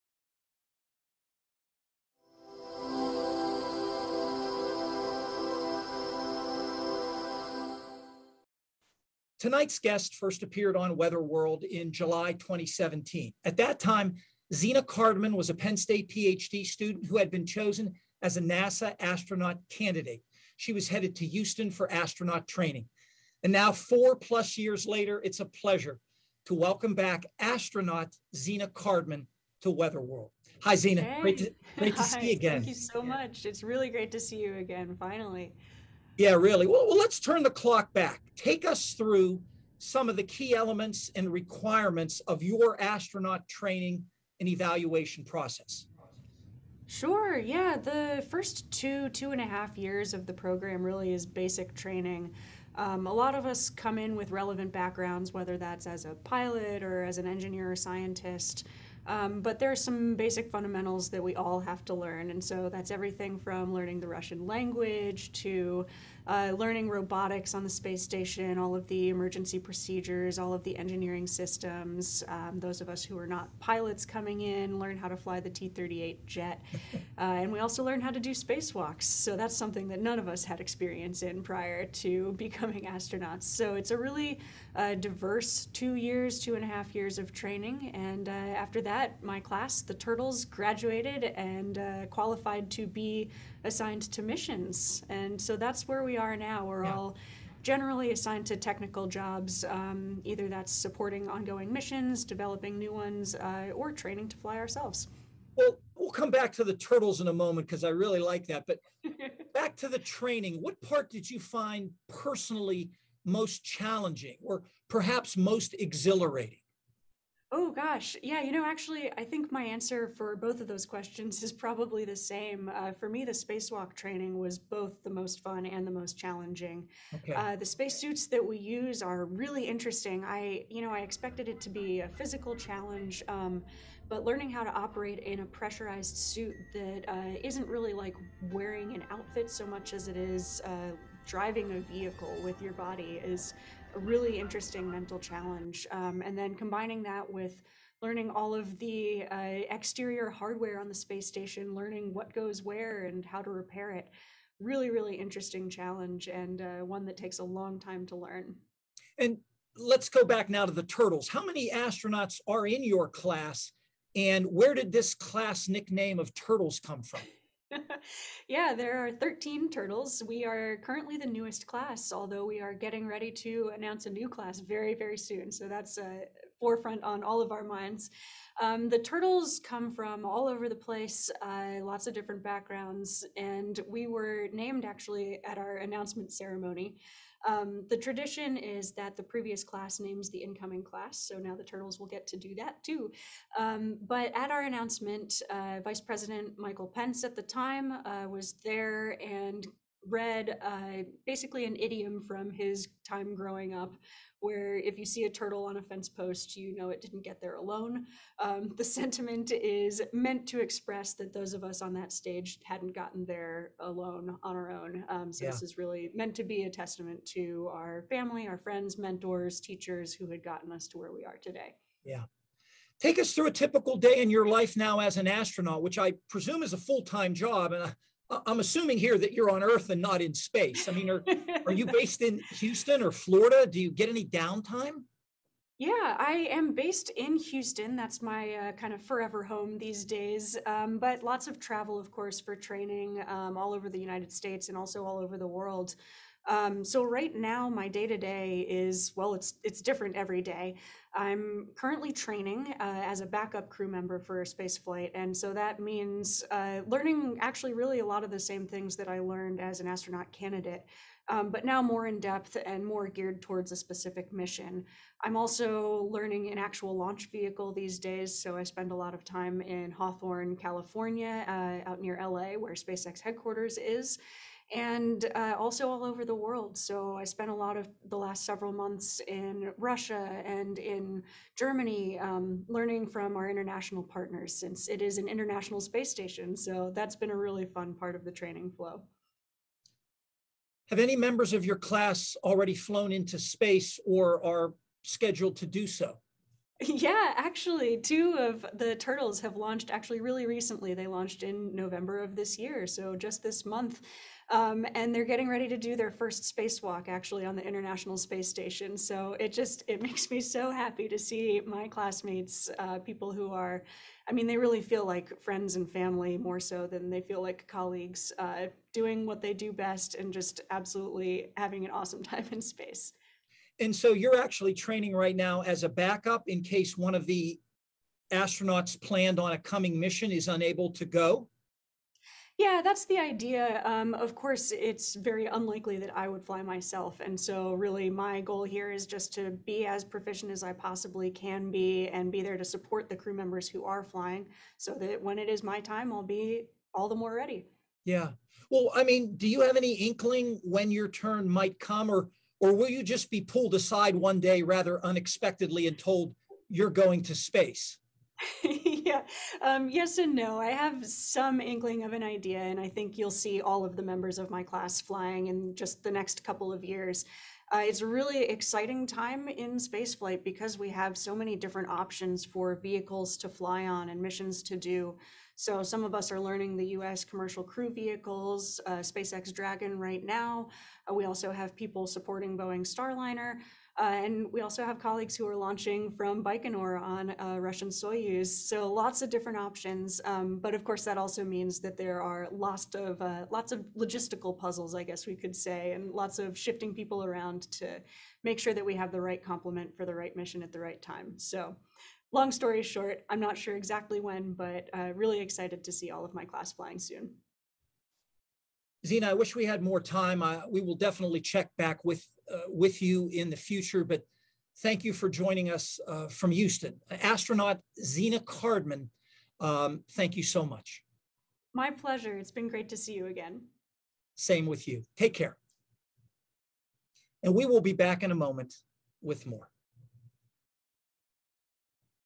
Interview with NASA Astronaut Zena Cardman
When we last interviewed Zena Cardman in July 2017, she was Penn State PhD student who had been chosen as a NASA Astronaut candidate. Catch up with her on her endeavors towards space exploration in this edition of Cosmic Connections.